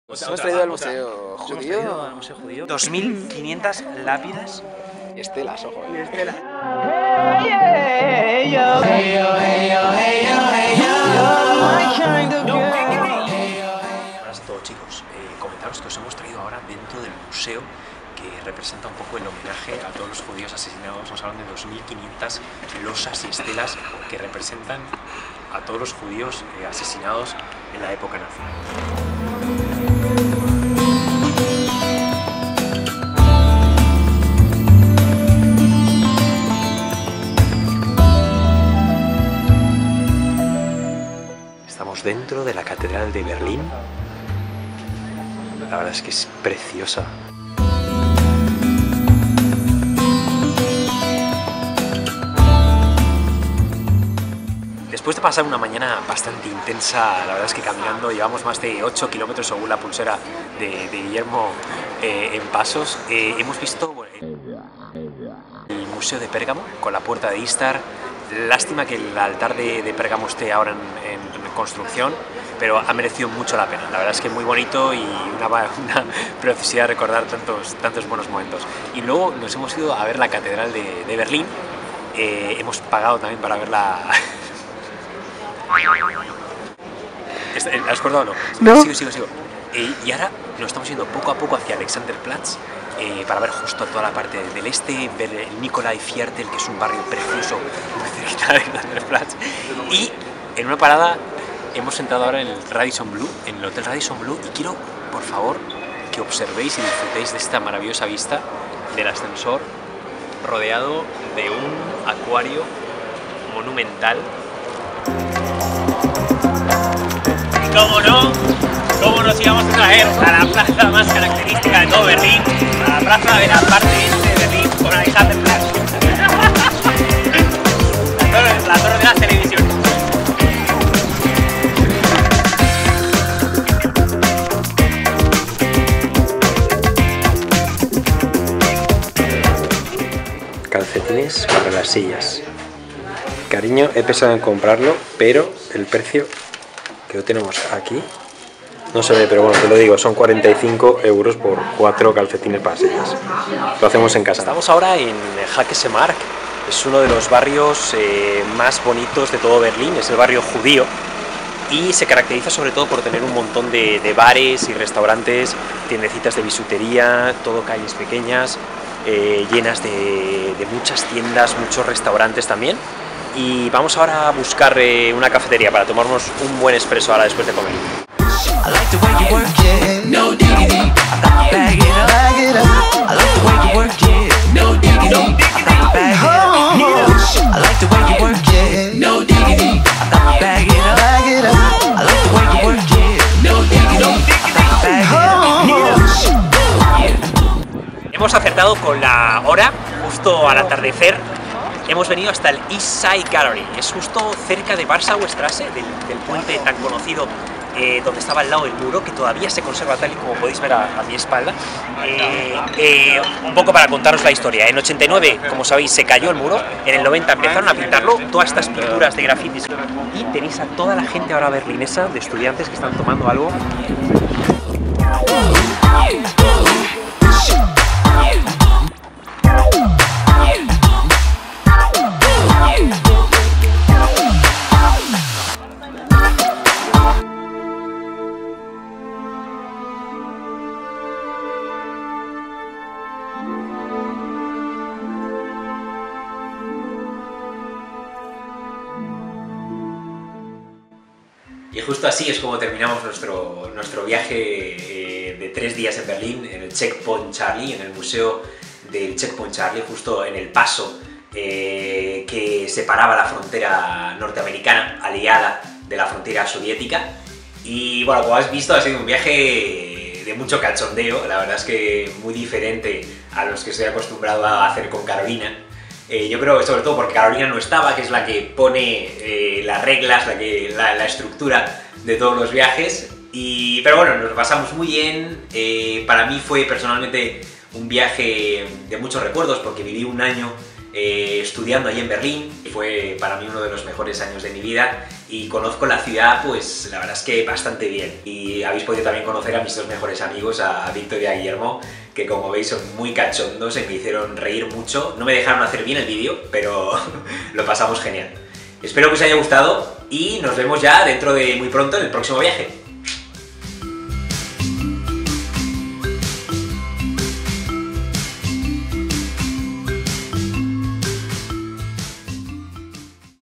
Fin de semana de chicos en Berlín - Alexander Platz, Monumento al Holocausto, Museo de Pérgamo...
Rafa se escapa a Berlín, ciudad en la que vivió un año como estudiante Erasmus, con sus dos mejores amigos. El resultado, como veréis, un vídeo que tiene de todo menos de serio y riguroso jajaja.